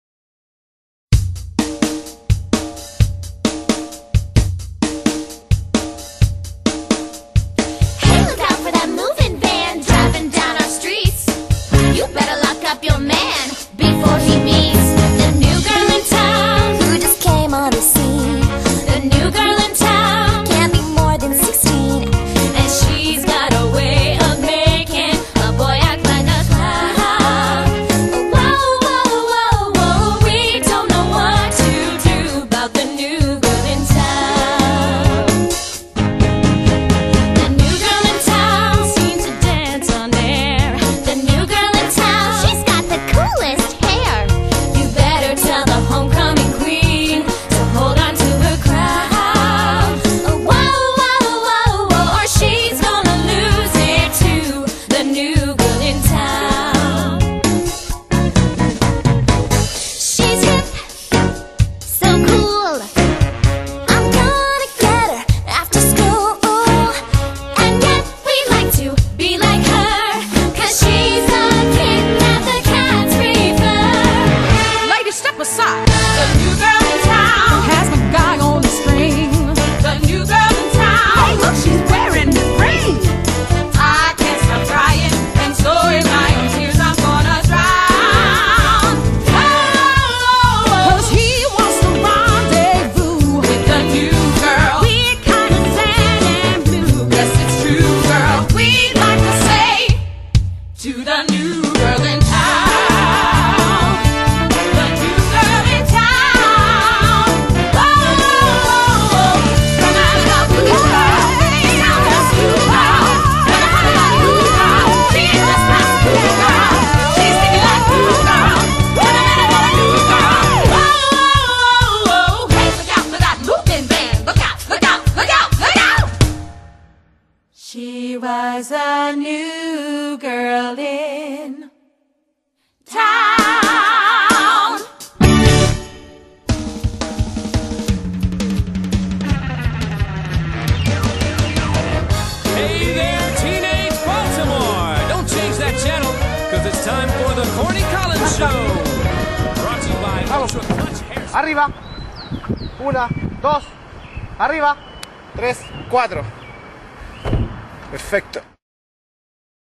The New Girl in Town - Golden Retriever
Dedicated to OLIVA, "The New Girl in Town".
(The recent Golden Retriever adopted, almost 3 months ago).